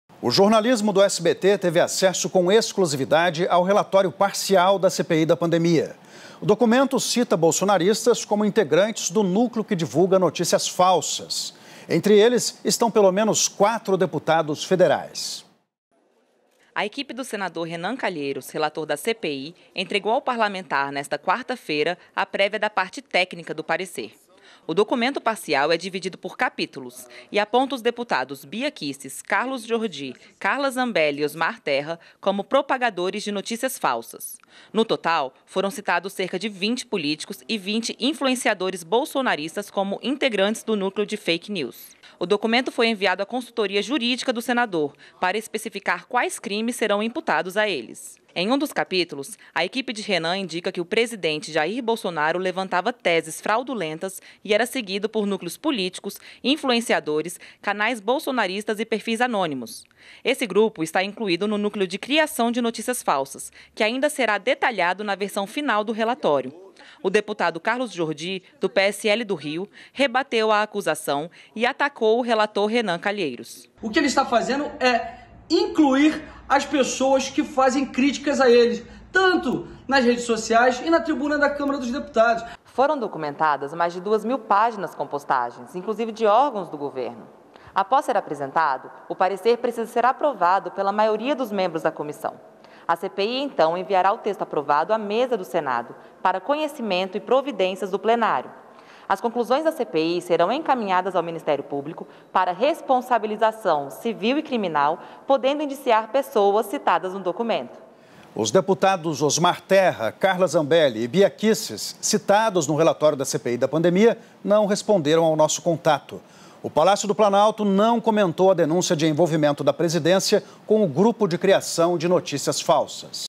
FUI INCLUÍDO NO RELATÓRIO DA CPI POR RENAN CALHEIROS
O SBT fez reportagem sobre o fato de Renan Calheiros ter me incluído no relatório da CPI da pandemia, mas omitiu minha fala toda. Minha resposta toda foi essa: “Isso já era de se esperar. Sempre disse que Renan Calheiros tinha seu relatório pronto com uma narrativa criminosa contra o Presidente Bolsonaro. O que ele fez foi apenas adicionar os nomes de deputados que o criticam. Fui e sempre serei um dos maiores críticos de Renan Calheiros nas redes e na tribuna da Câmara dos Deputados. Com dezenas de inquéritos e ações penais pelos mais diversos crimes, ele não é digno de ser Senador muito menos de ser relator de qualquer CPI. Ele deveria estar preso!”.